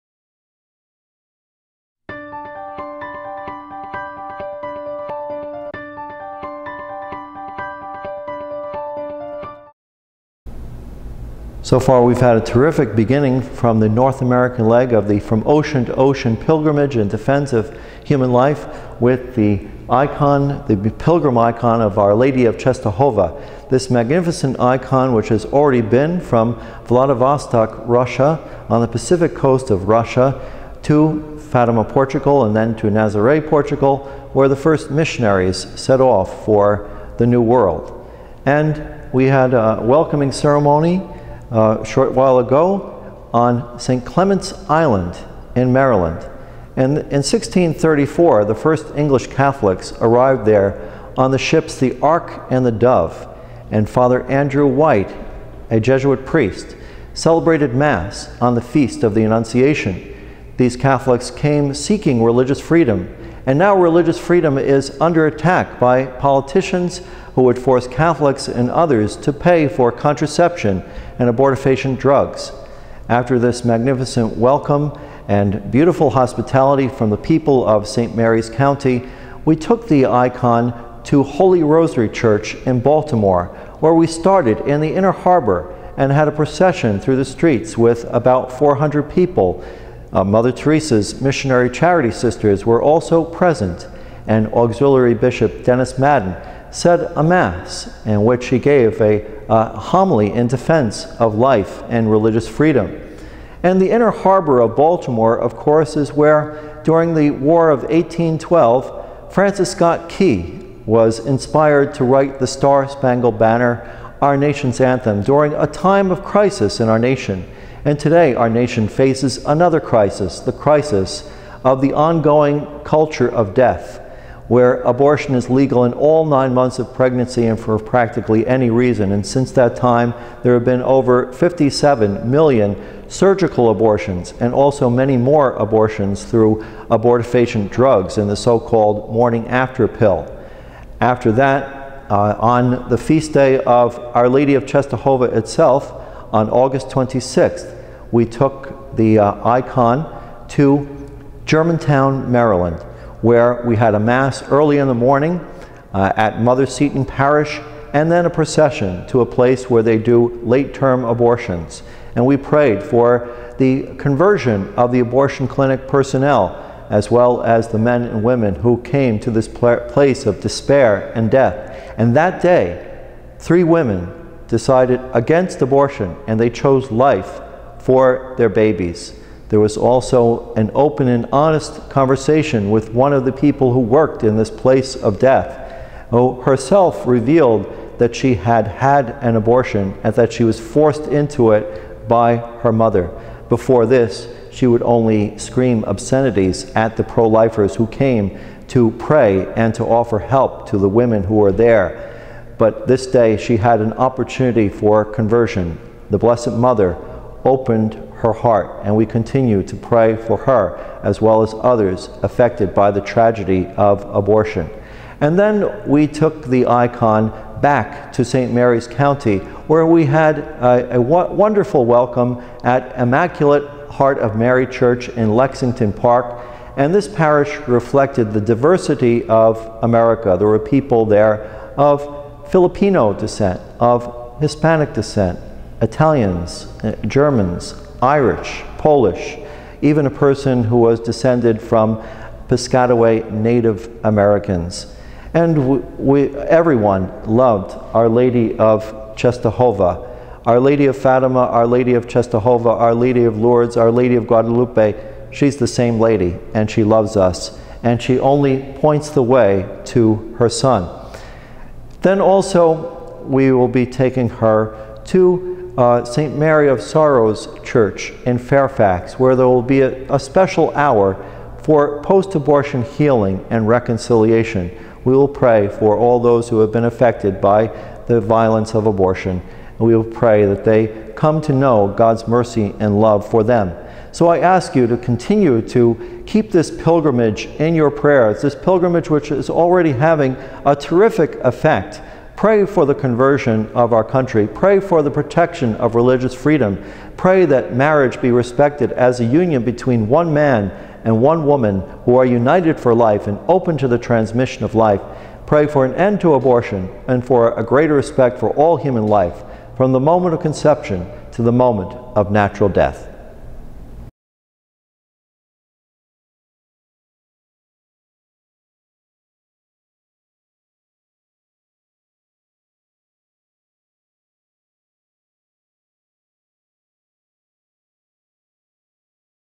Fr. Peter West Gives Update on Ocean to Ocean Pilgrimage
Human Life International Vice President for Missions Father Peter West is spending the next year traveling across North America with the icon of Our Lady of Czestochowa. The From Ocean to Ocean campaign in defense of life is already having a positive affect in the U.S. since the North American leg of the journey began in August 2013. Learn more about the pilgrimage and how you can support HLI's efforts